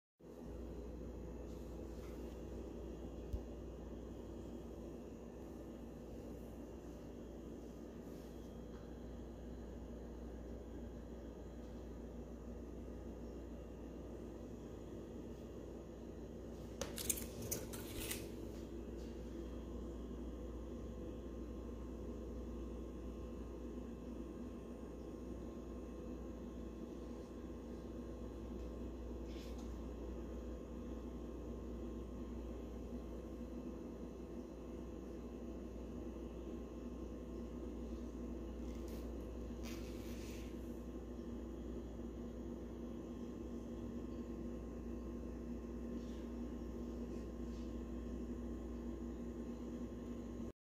Deer Skull Dip Results
So since I couldn't post pictures, here is a short video of the outcome of my first dip.